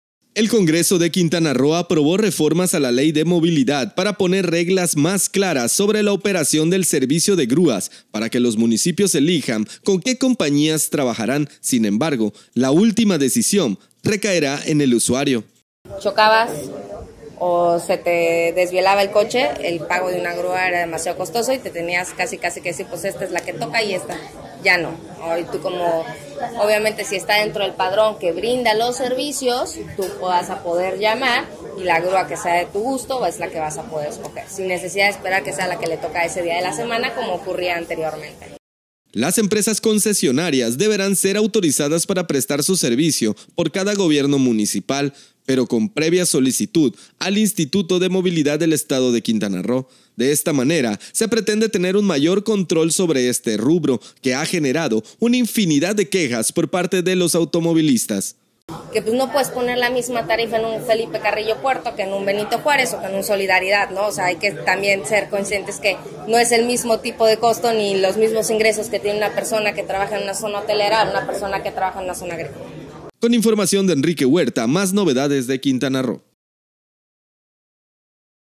Así operarán grúas en los municipios de Quintana Roo tras reforma
El Congreso de Quintana Roo reformó la Ley de Movilidad del Estado, en la que otorgan la responsabilidad a los 11 municipios de la Entidad para decidir con qué compañías de grúas pueden laborar.

Asimismo, la forma con la que operarán en territorio estatal también fue modificada.